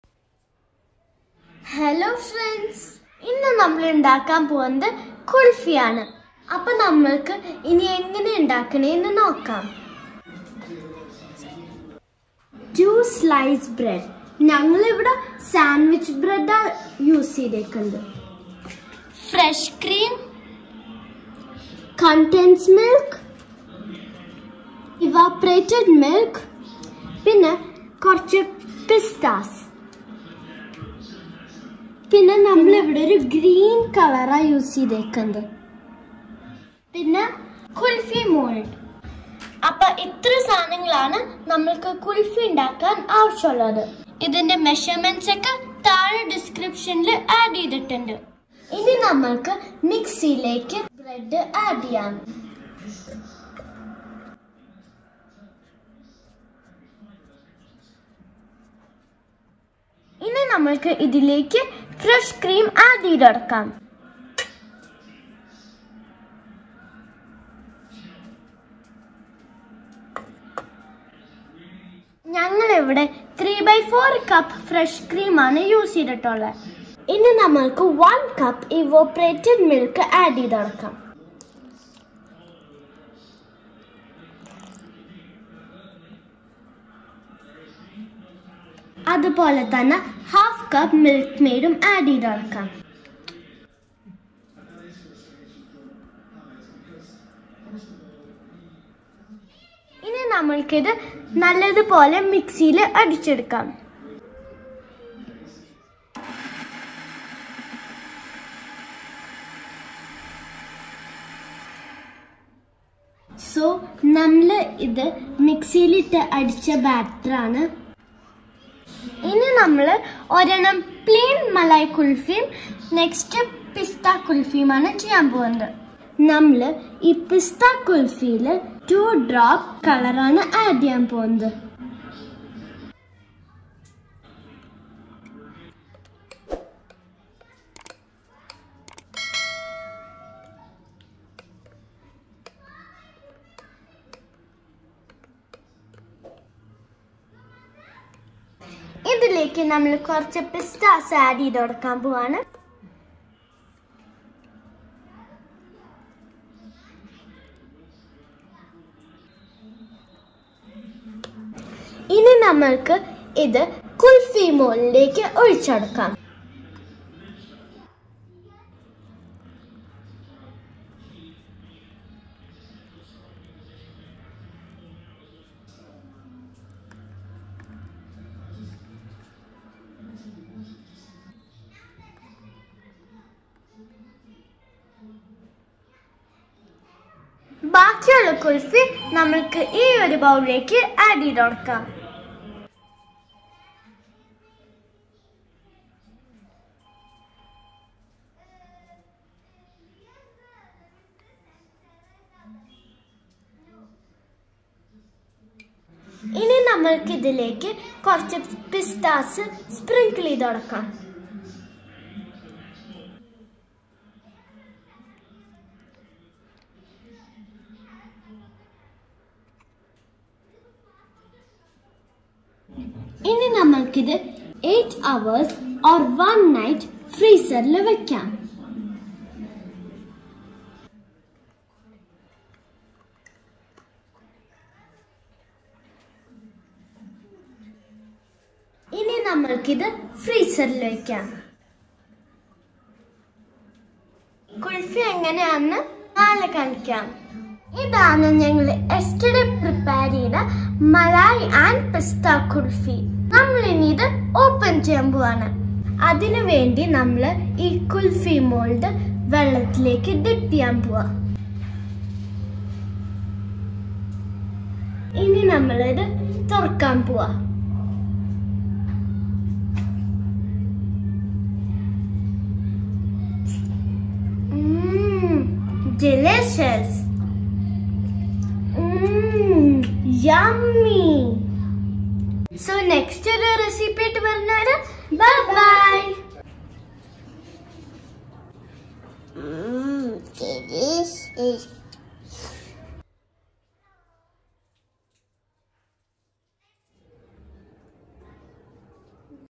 Easy Kulfi Recipes | Malai & Pista | Bread Kulfi | ബ്രെഡ് ഉണ്ടോ? രുചികരമായ കുൽഫികൾ തയ്യാറാക്കാം
Delicious & easy kulfi recipe.

Ingredients
Bread - 2 nos
Fresh cream - 3/4 Cup
Condensed milk - 1/2 cup ( adjust according to taste)
Evaporated milk - 1 cup (2 cup full fat milk reduced to 1 cup, i added 1/2 cup sugar & cooked in                  preasure cooker, for the colour. After that reduced to 1 cup)
Sliced Pistachios - For garnishing